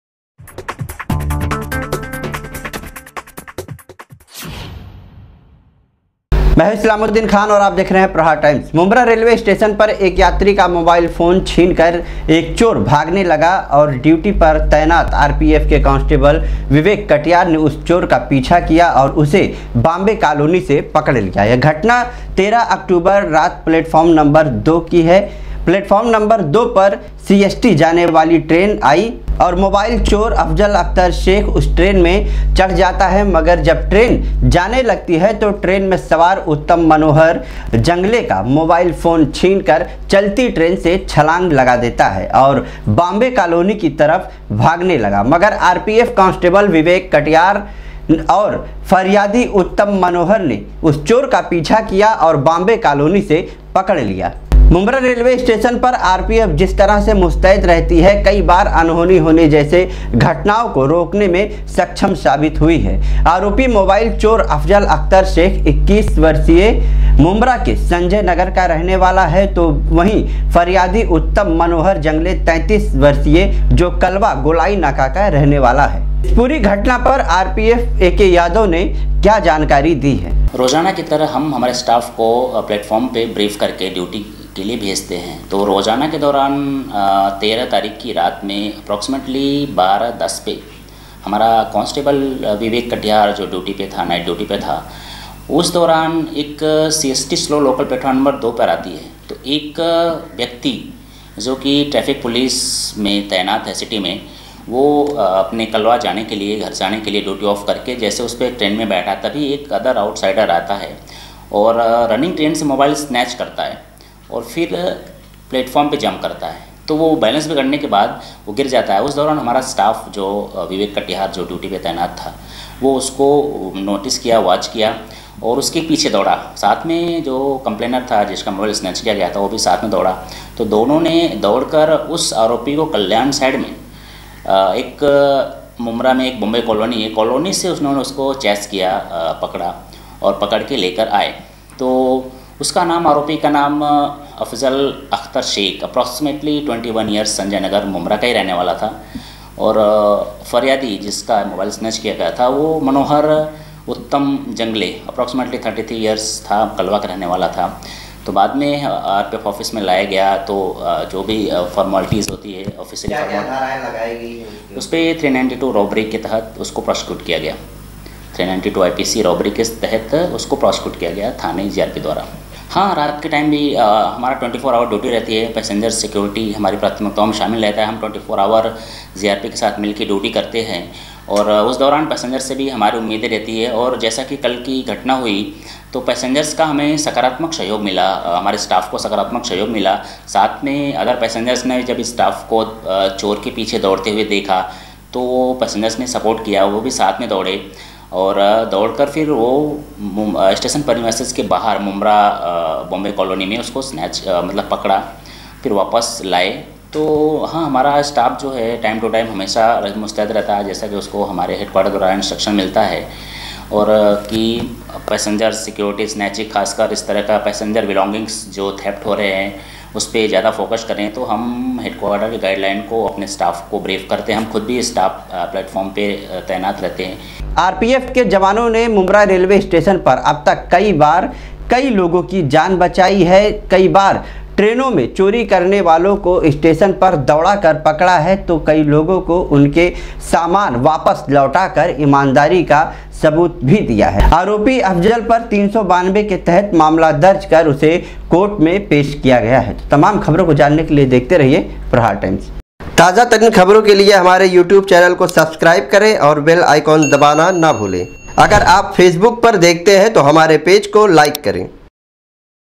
चलती लोकल ट्रेन में छीना मोबाइल आरोपी को RPF ने किया गिरफ्तार | Aapka Prahar Times -
चलती लोकल ट्रेन में छीना मोबाइल,आरोपी को RPF ने किया गिरफ्तार |islamuddin khan
लोकल ट्रेन में मोबाइल छीन लगाई छलांग

RPF कोस्टबल ने आरोपी को पकड़ा

आरोपी मुम्ब्रा संजय नगर का रहने वाला है